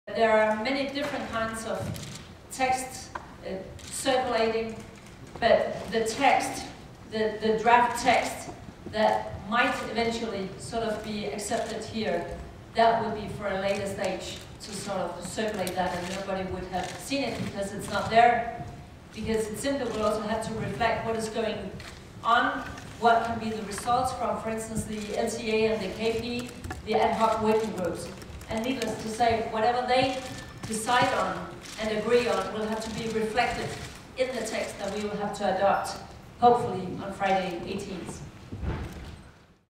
Connie Hedegaard
Lors de la conférence de presse douverture des négociations, la très charismatique présidente danoise de la Cop15 tentait de convaincre les journalistes quil nexiste pas de texte déjà rédigé.